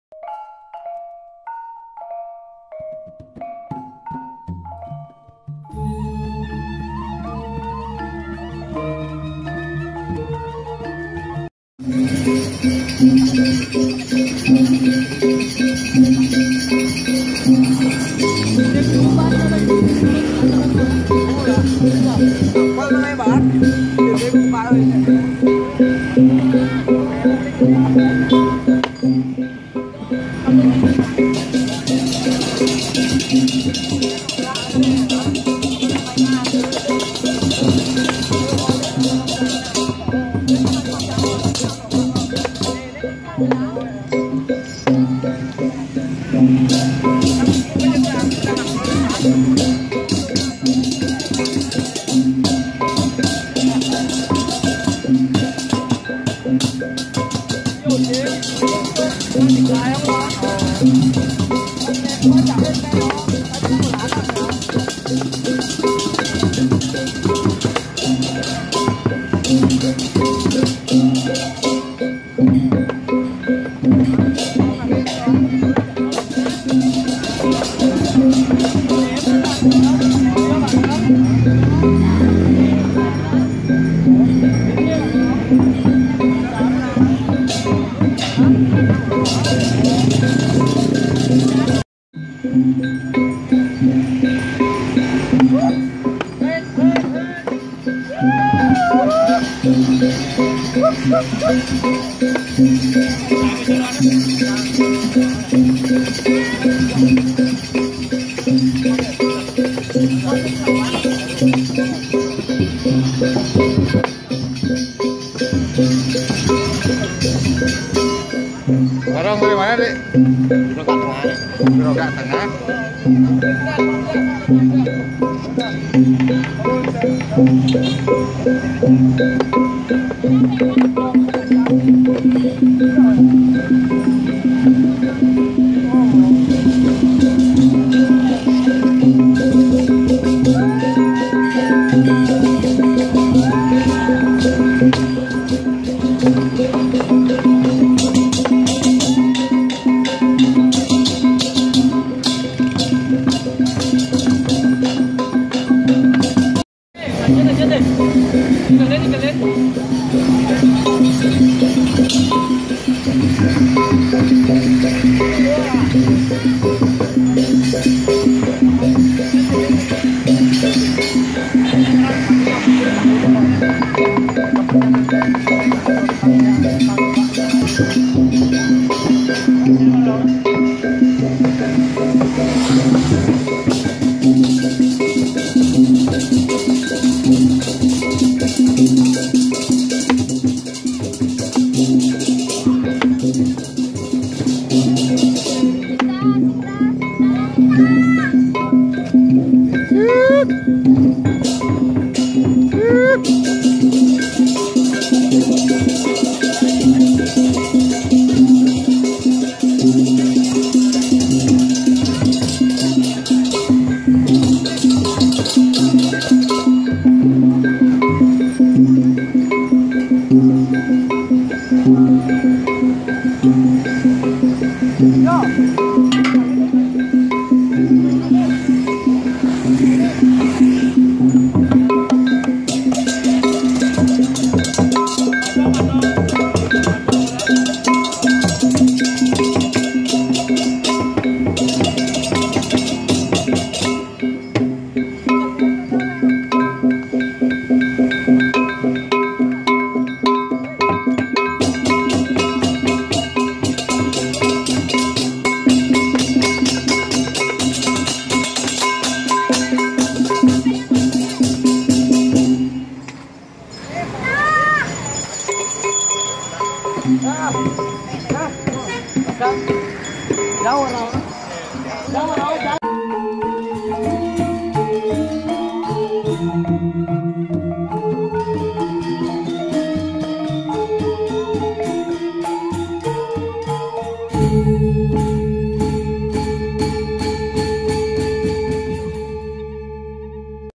NEW 2020 !!! BARONG BANGKUNG MEPETUK GROGAK TENGAH TABANAN.
Barong Ngelawang merupakan salah satu seni tradisi bali yang sangat digandrungi oleh masyarakat bali khususnya kaum muda mudi.Tradisi ngelawang ini biasanya dilakukan saat menjelang hari raya Galungan dan sampai hari Raya Kuningan.Barong dipentaskan keliling Desa oleh sekaa Barong yang diiring Tabuh yang sangat energik sehingga menimbulkan suasana kegembiraan.